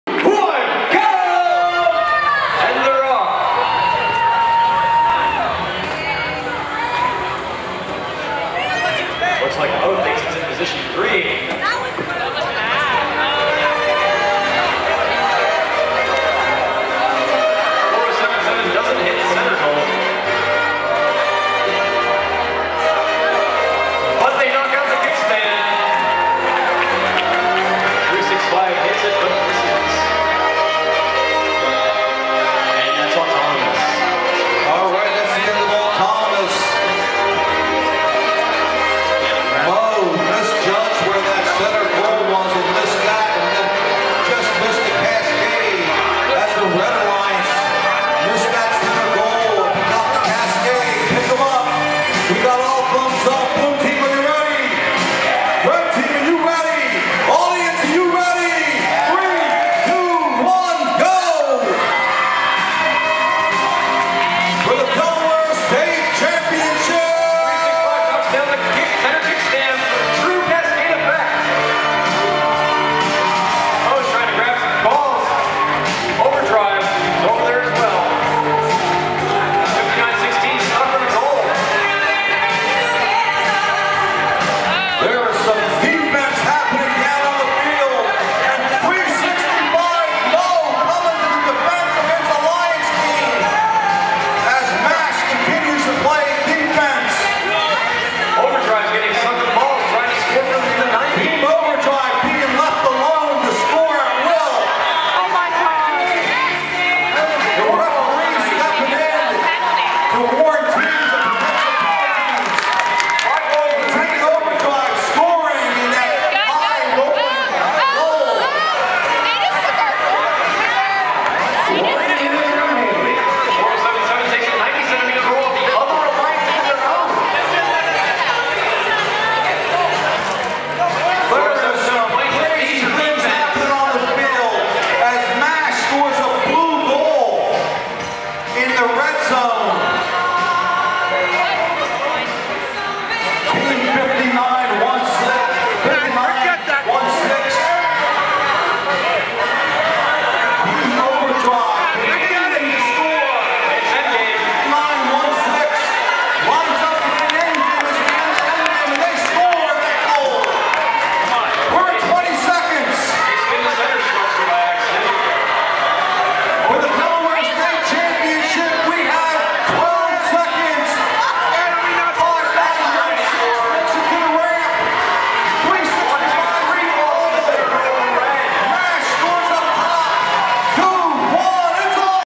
Delaware State 2015 FTC Championship Final 3 - MOE
1/31/2015 Final - Match 3
Blue 440 (365,5916) Red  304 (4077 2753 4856)
Delaware State University